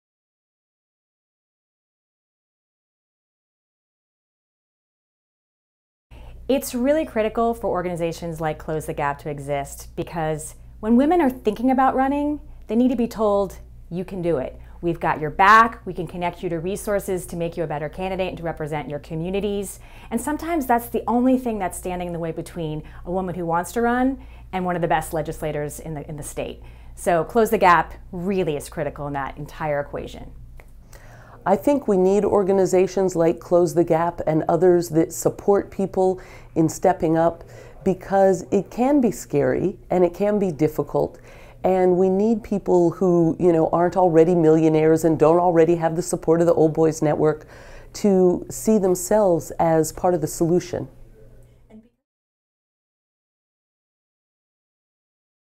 Why is CTGCA important?
Rebecca Kaplan (Oakland city council member) and Noreen Farrell (Executive Director of Equal Rights Advocates) discuss why organizations like Close the Gap are important in helping to reach gender parity in the legislature.